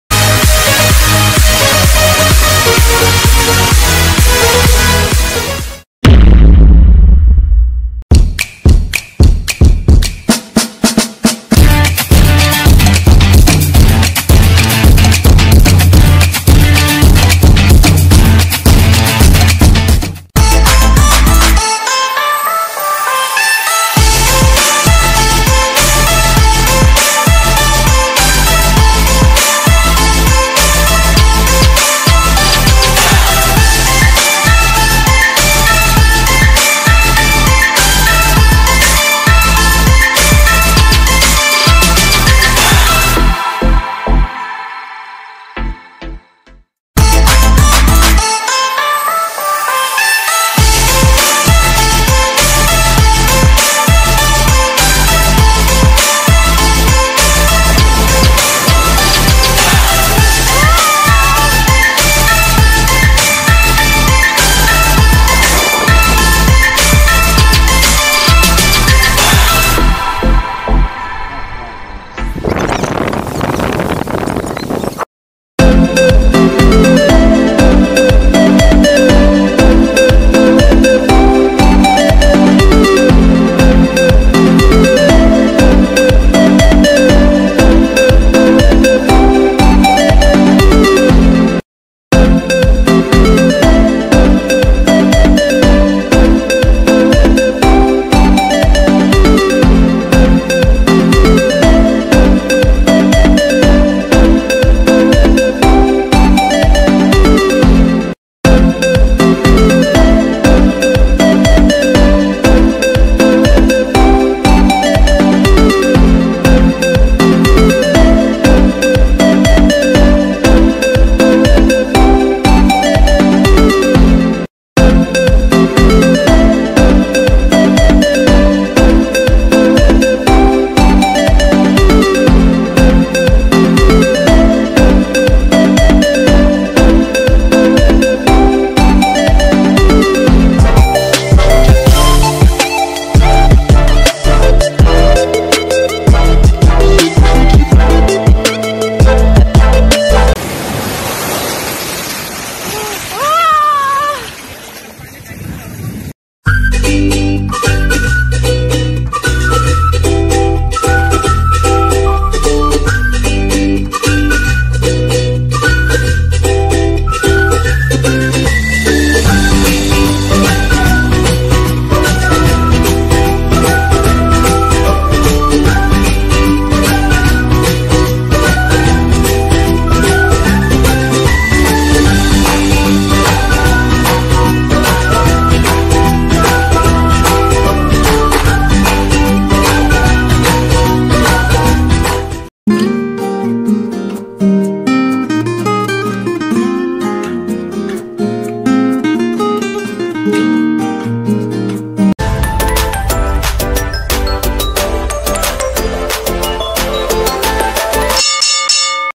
MANANOY BEACH / VANISHING ISLAND or WARA-WARA BEACH / Two in One Destination
TWO IN 1 DESTINATION for Island hopping and summer getaway : MANANOY BEACH and VANISHING ISLAND

MANANOY BEACH is a newly developed and best distination for famiy and friends getaway . It is also recommend if you are looking for an island hopping. Located at Malictay , San Miguel Island Tabaco Albay.

VANISHING ISLAND or WARA WARA BEACH- Enjoy  the Vanishing Island of Malilipot Albay.

with
Floating  Cottage, kayaking and
Island Hopping